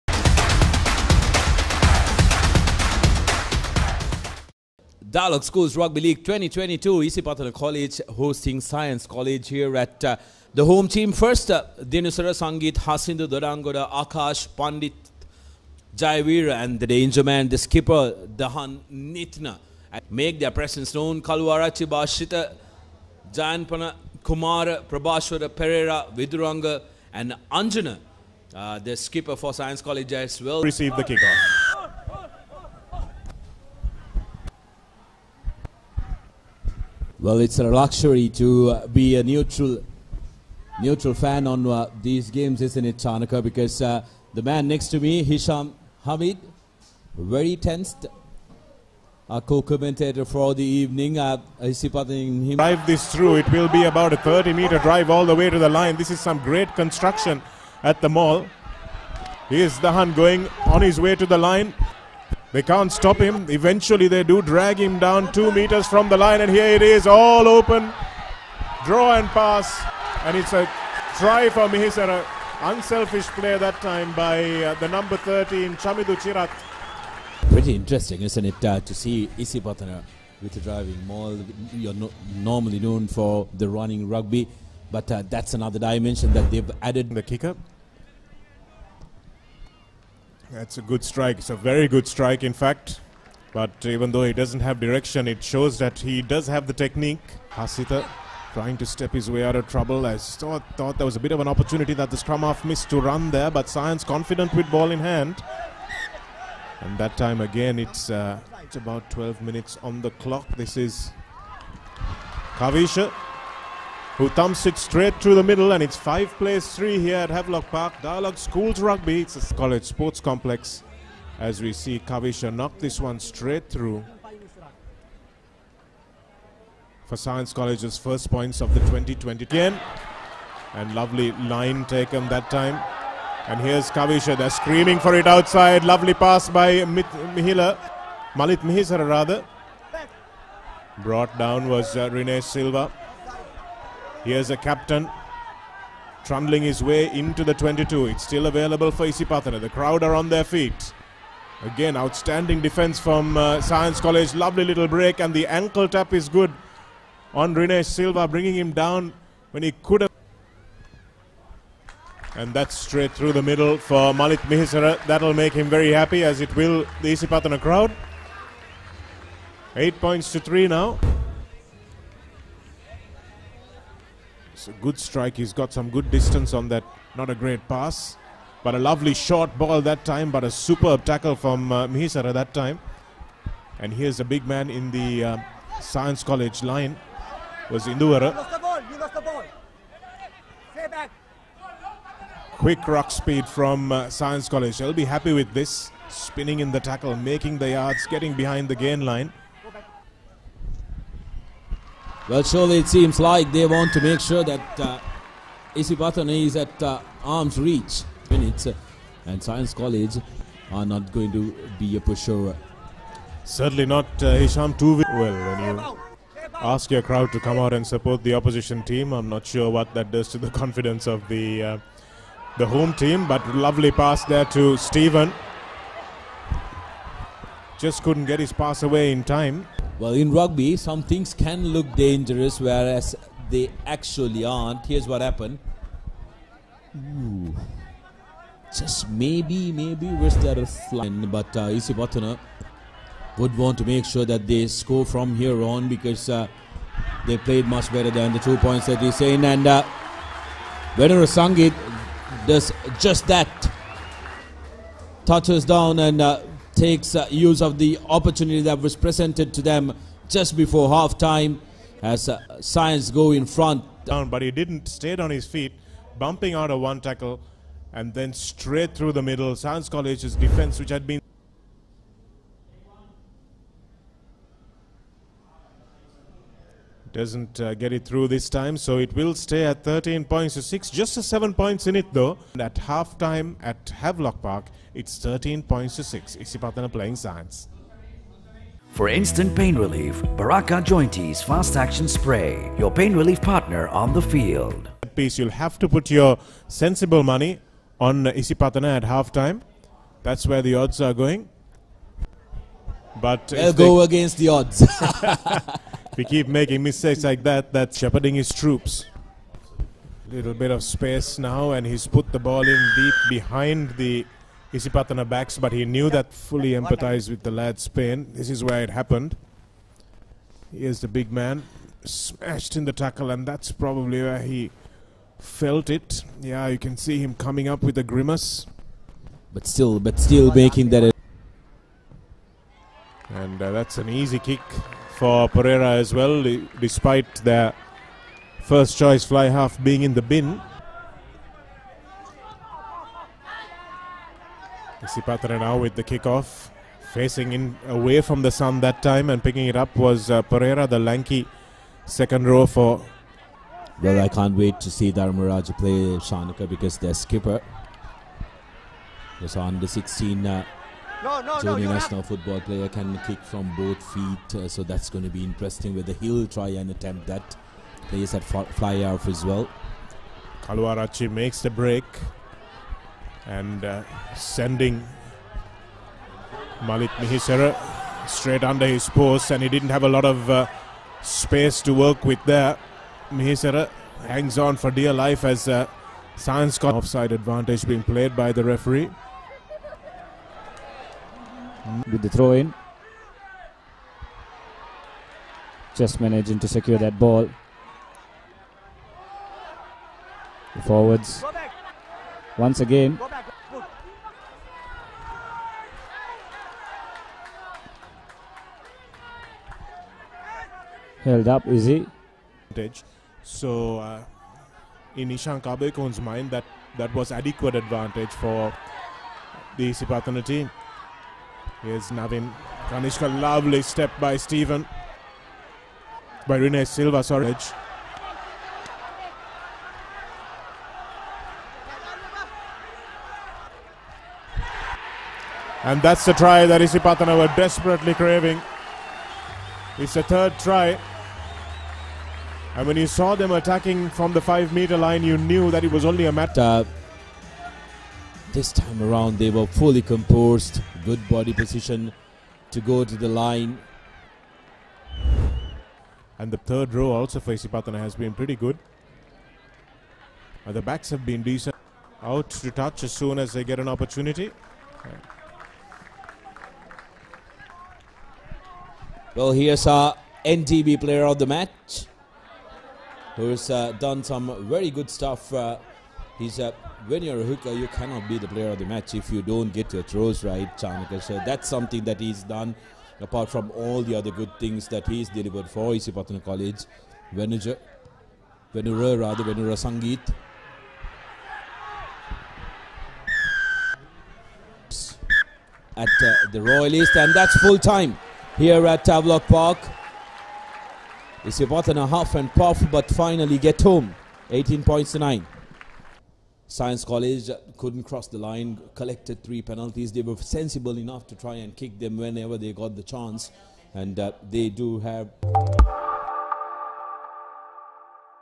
HIGHLIGHTS - Isipathana College vs Science College
The Highlights from the Isipathana College vs Science College match in the Dialog Schools Rugby League 2022 played on the 23rd of June at Havelock Grounds, Colombo.